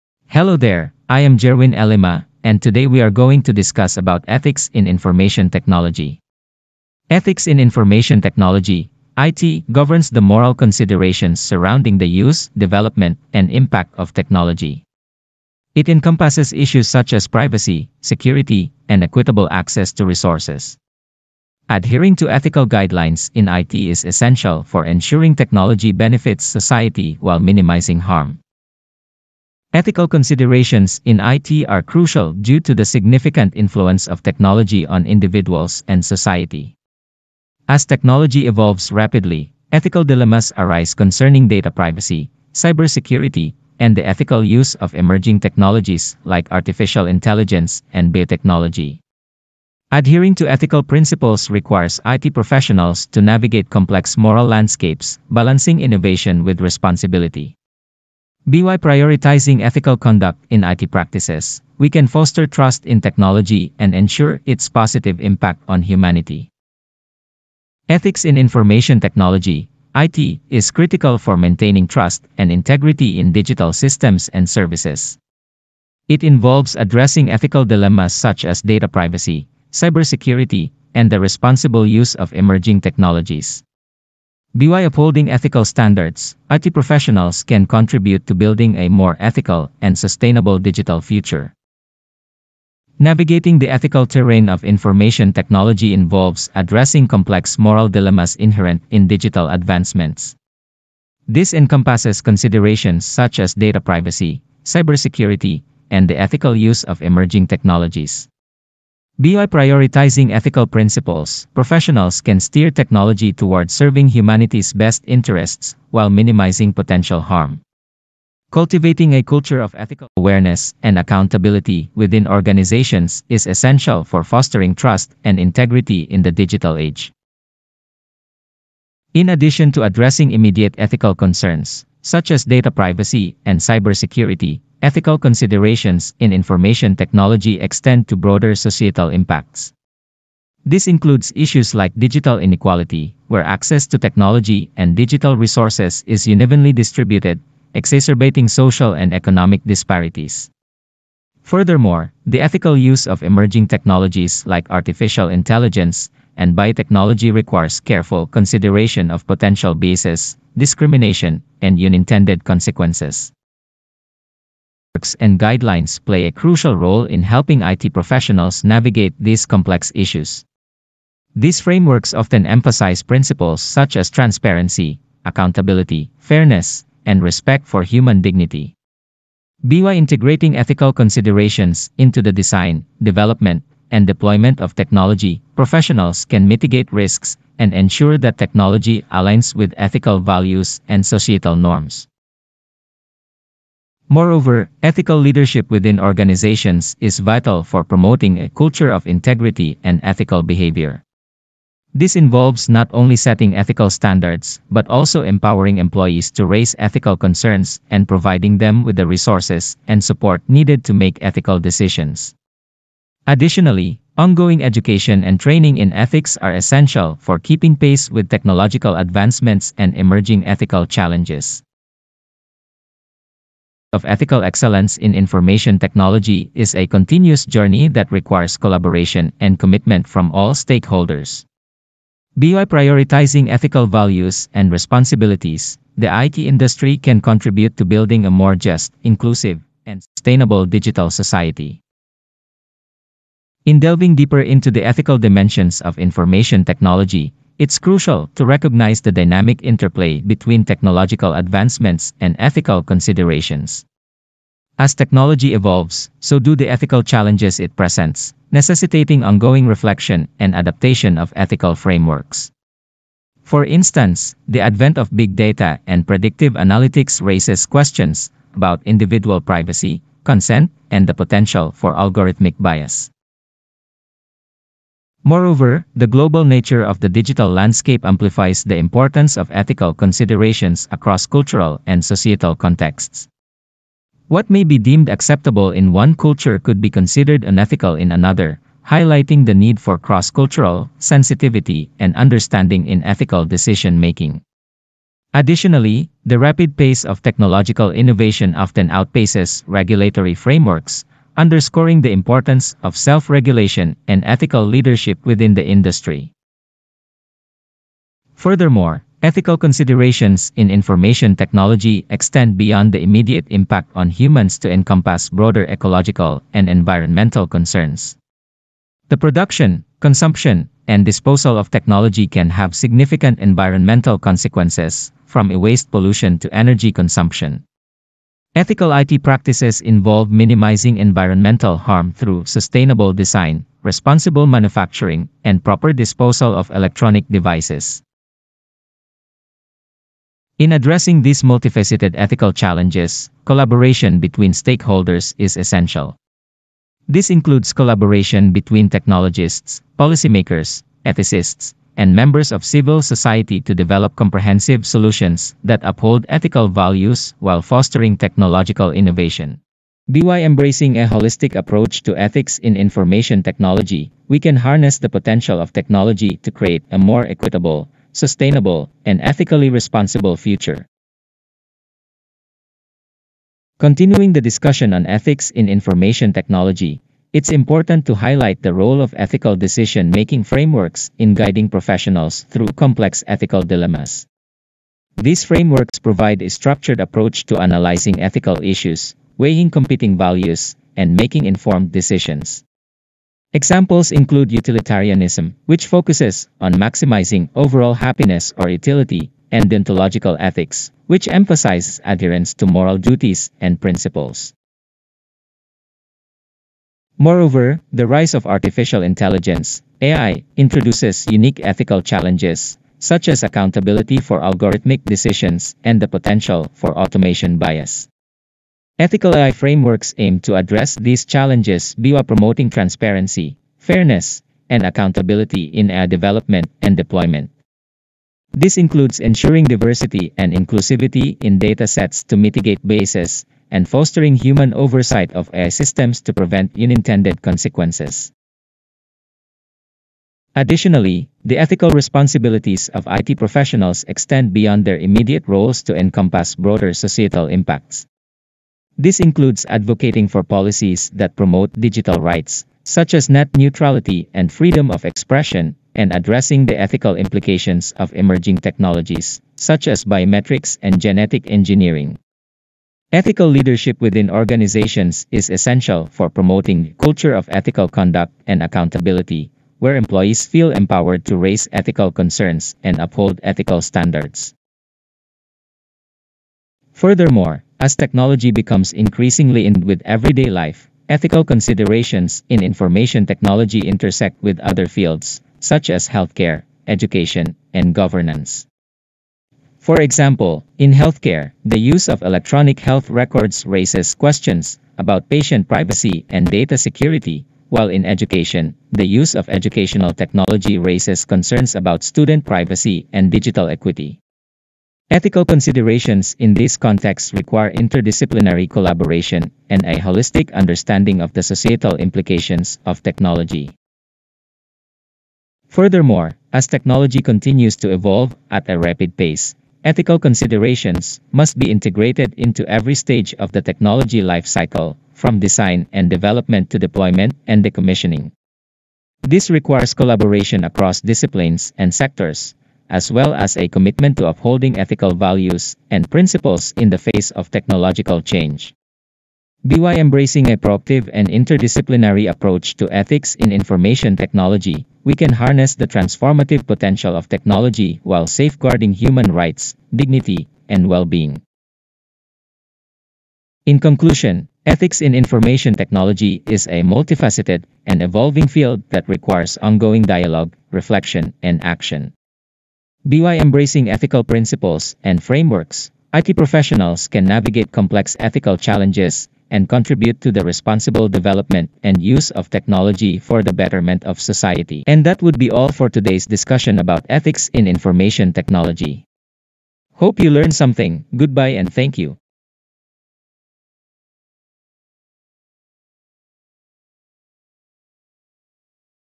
Social and Professional Issues in IT - Ethics In Information Technology
TITLE:
"ETHICS IN INFORMATION TECHNOLOGY"

DESCRIPTION:
"Information technology ethics is the study of the ethical issues arising out of the use and development of electronic technologies. Its goal is to identify and formulate answers to questions about the moral basis of individual responsibilities and actions, as well as the moral underpinnings of public policy. Ethical handling of such data is essential to protect users from potential harms like identity theft, reputation damage, and financial losses.

SOFTWARE THAT HAS BEEN USED:
CHAT GPT
WONDERSHARE FILMORA9
ANIMAKER